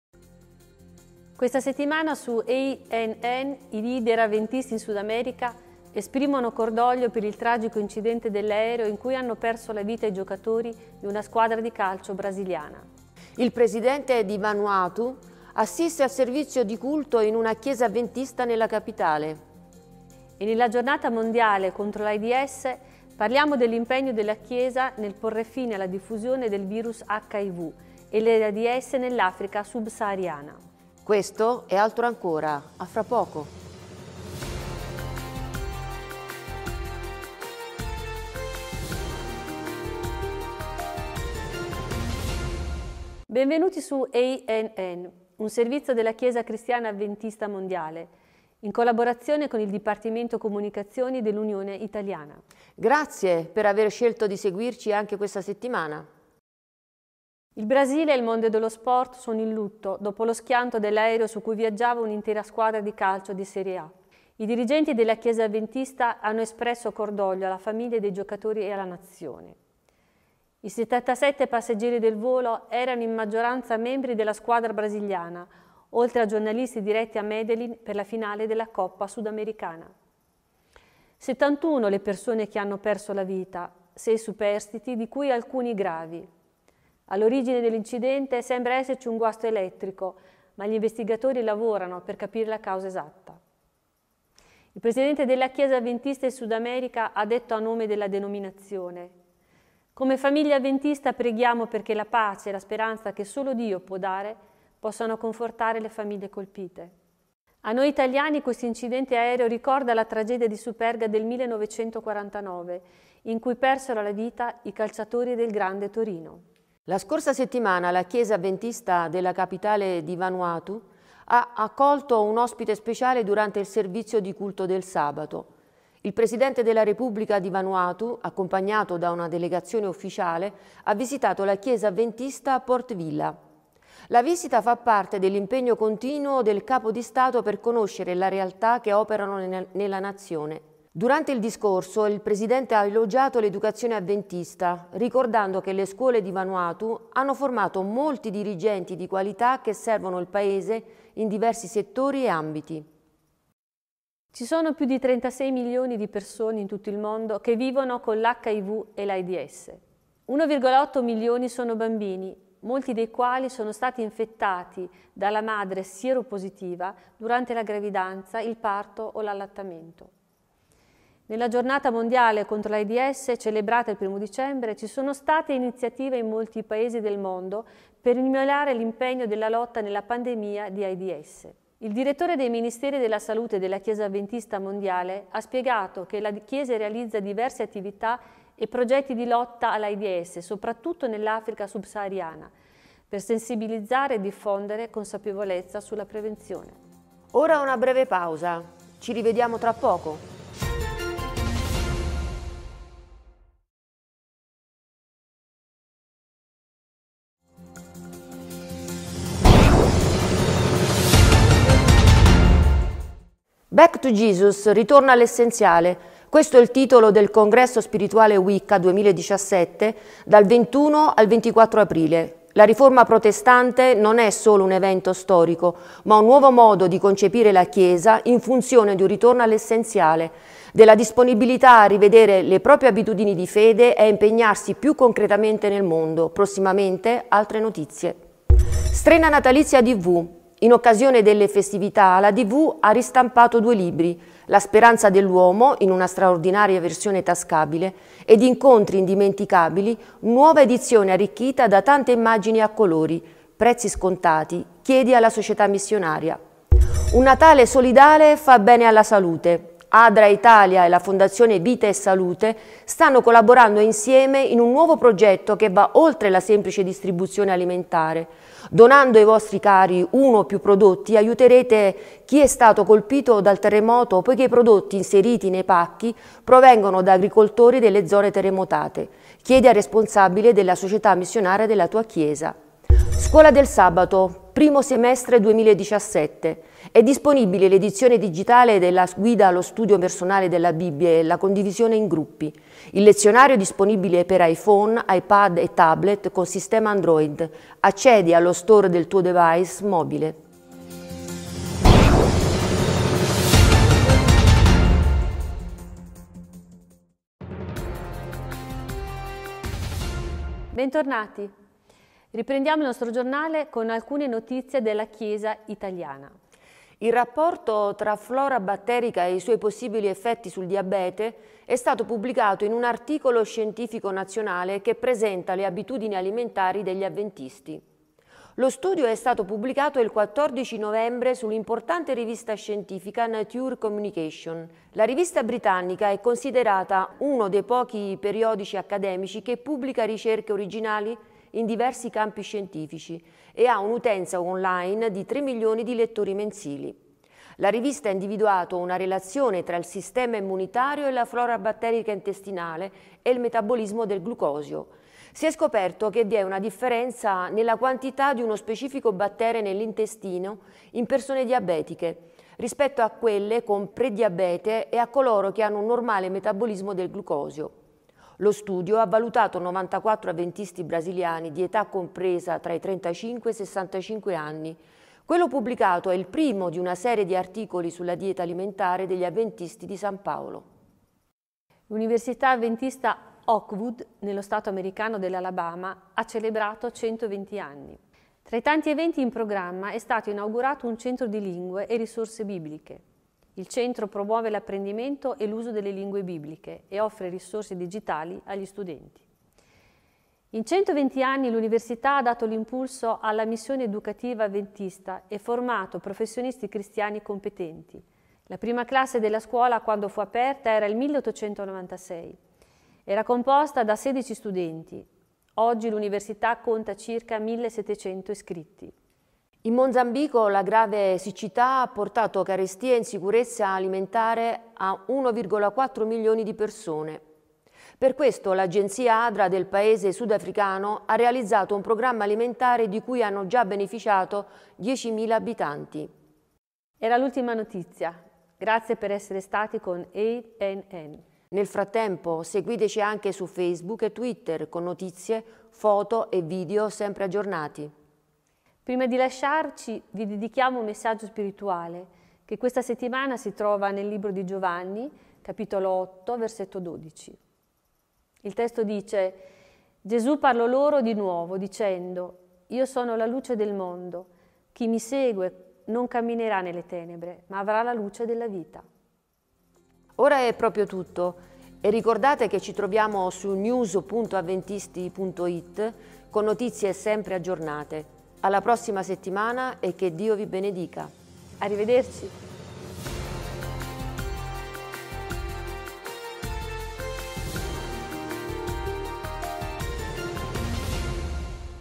Video giornale 2 dicembre 2016
Video giornale dal mondo avventista internazionale ed italiano.
Questa settimana su ANN:
I leader avventisti in Sudamerica esprimono cordoglio per il tragico incidente dell’aereo in cui hanno peso la vita i giocatori di una squadra .